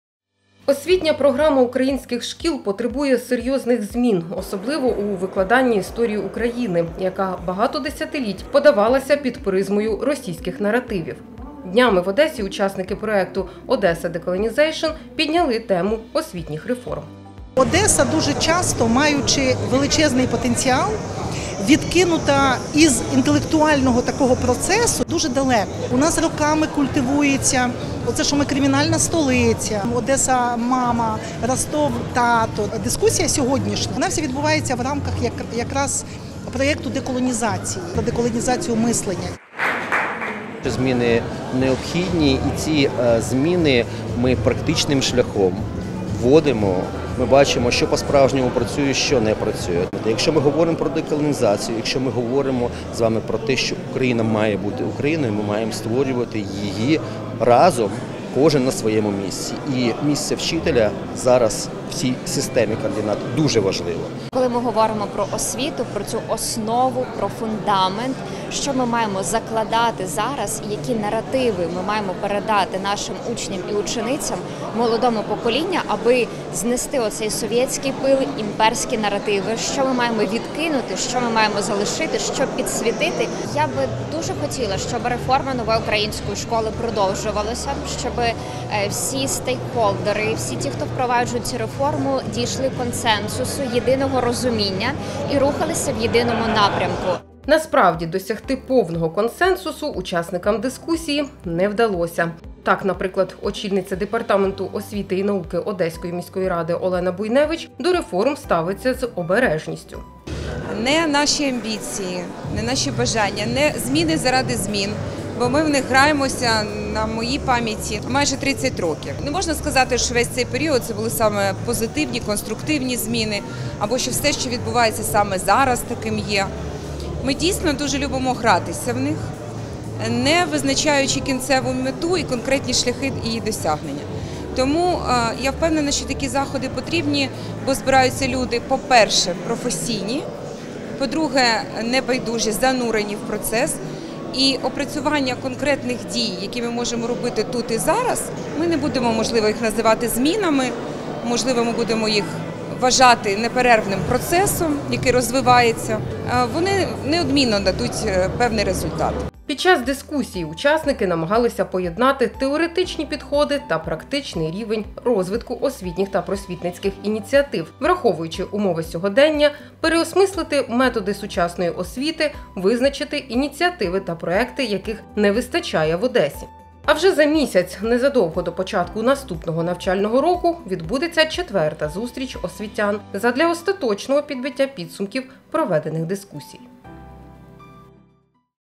Освітня реформа - вимога часу
В рамках проекту «Odesa Decolonization» в Одесі пройшла дискусія на тему освітніх реформ, учасниками якої стали практикуючі вчителі, представники влади, історики та просто небайдужі містяни, які мали можливість висловити власну думку з приводу нововведень та змін у програмі Нової української школи (НУШ). Без суперечок не обійшлося.